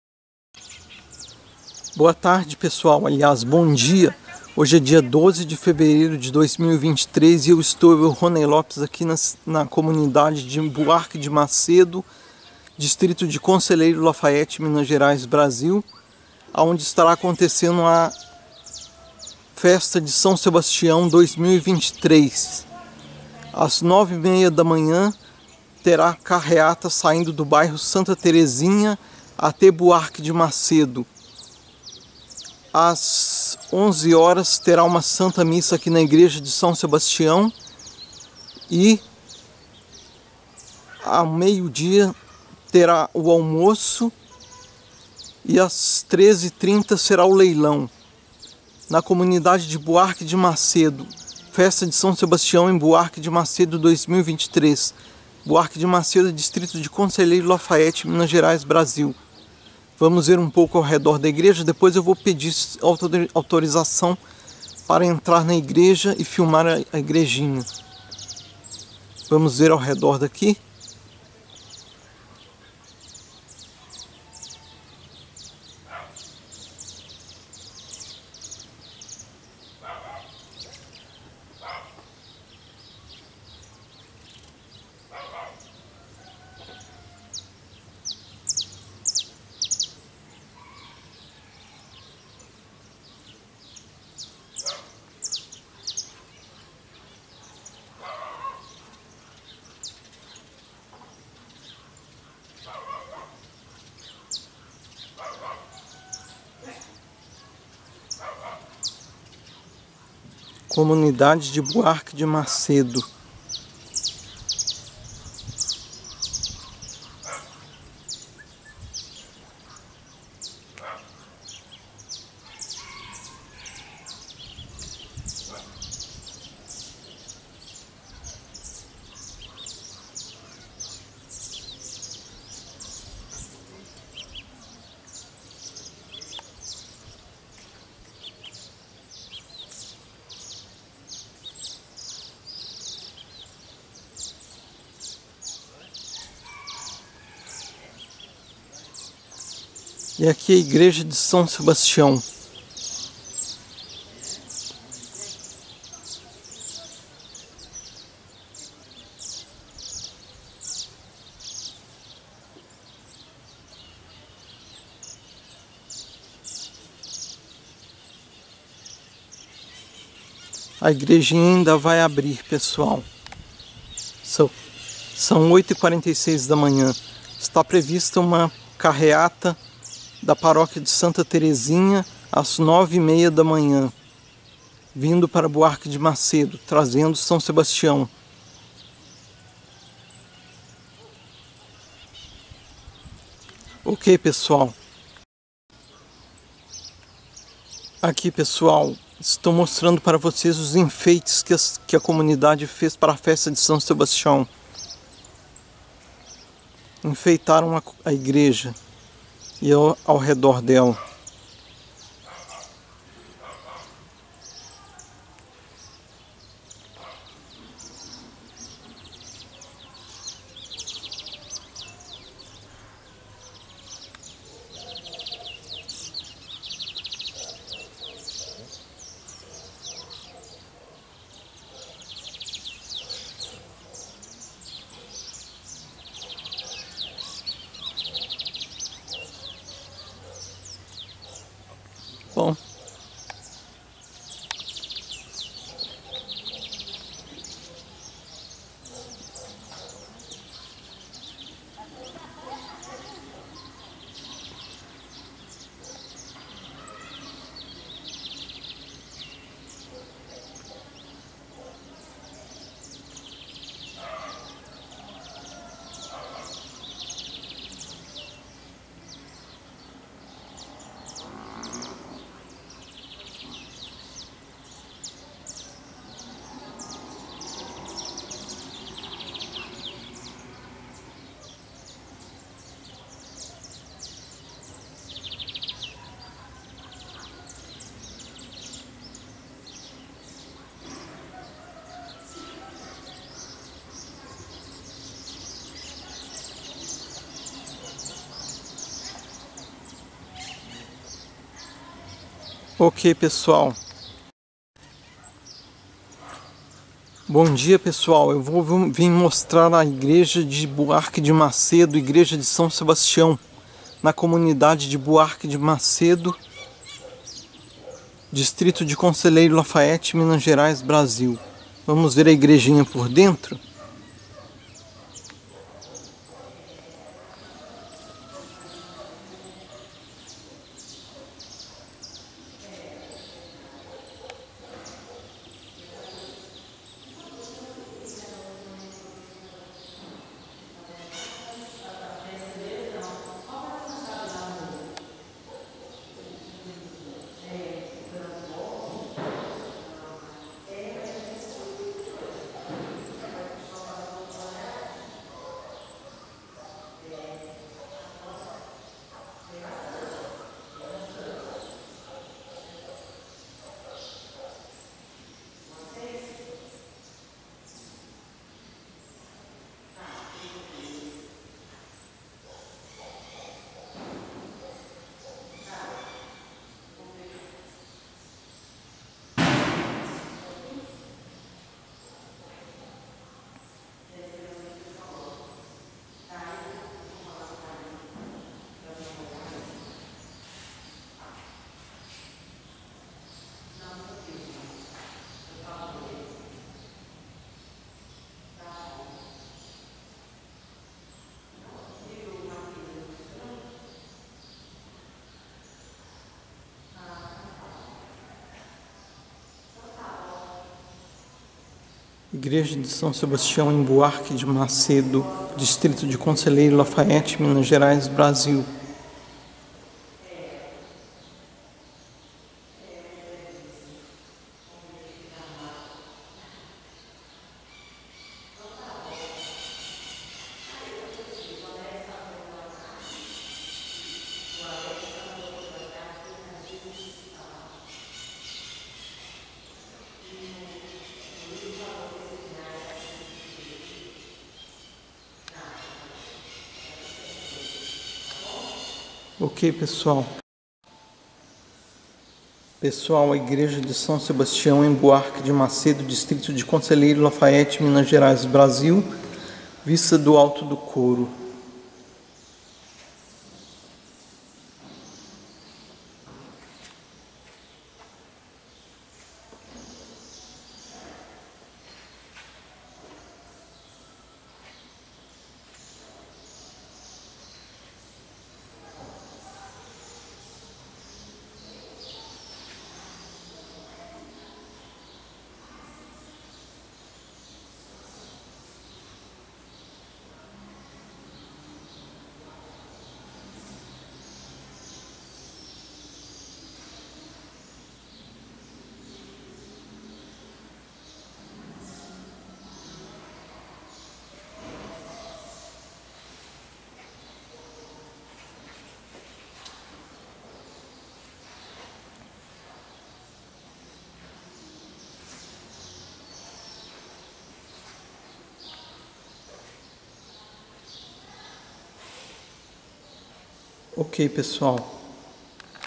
1-Festa de São Sebastião 2023 em Buarque de Macedo - Conselheiro Lafaiete, Minas Gerais, Brasil.
Gostou do vídeo? E ative as notificações. Festa de São Sebastião 2023 em Buarque de Macedo - Conselheiro Lafaiete, Minas Gerais, Brasil. Parte 1. Conhecendo a localidade, e a igreja por dentro e por fora. Os leilão ao final da festa tem a finalidade de arrecadar fundos para a reforma e pintura da fachada da igreja.